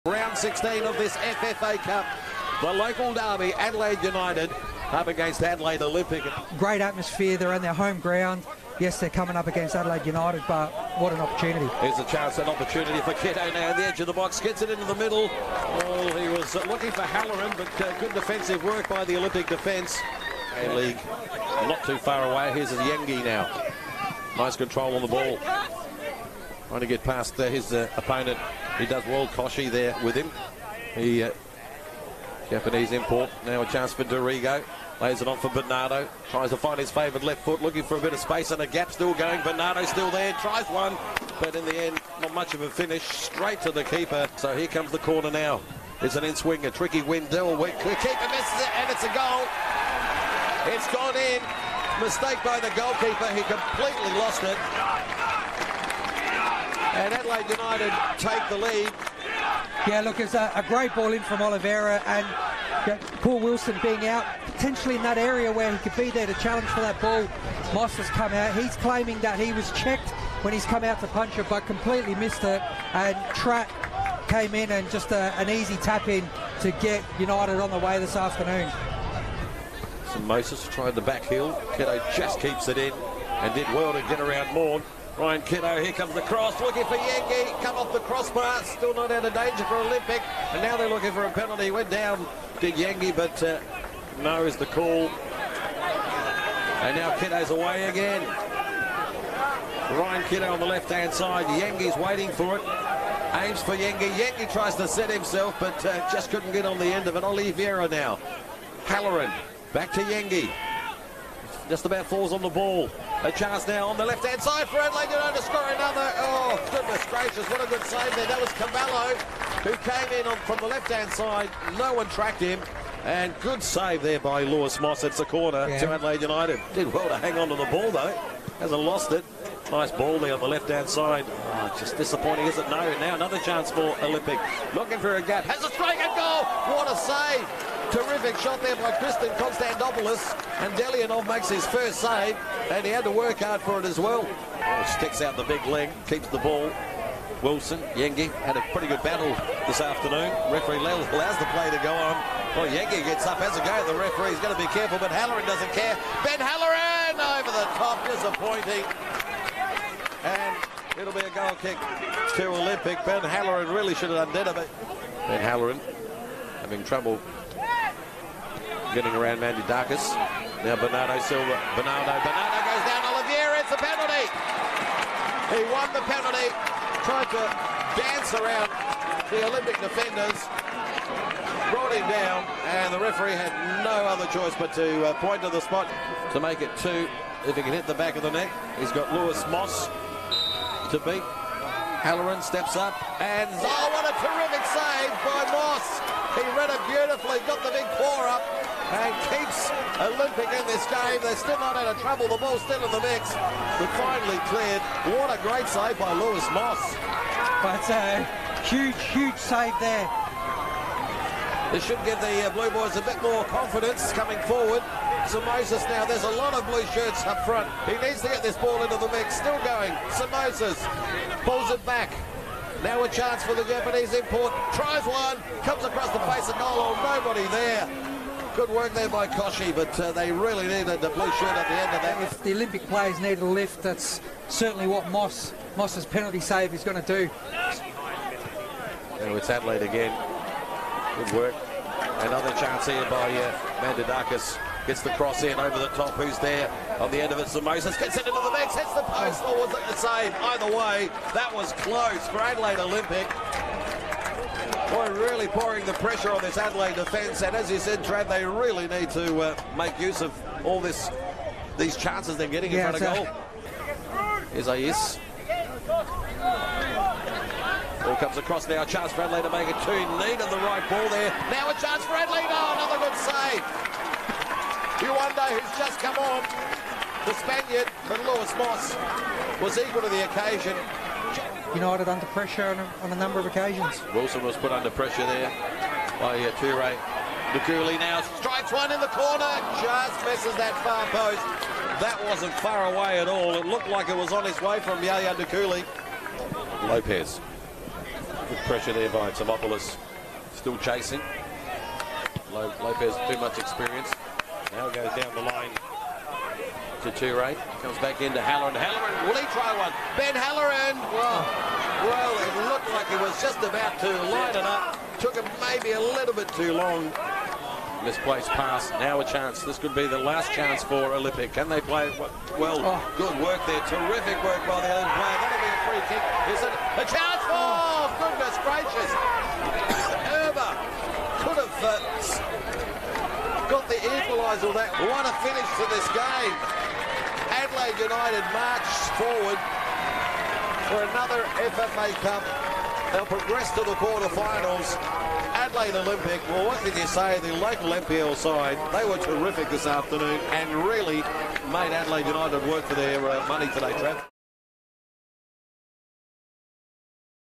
FFA Cup Highlights: Adelaide Olympic v Adelaide United
Jacob Tratt’s 16th minute goal – his first for the Club – was the deciding factor between the two sides, although the Reds had chances to put the game to bed.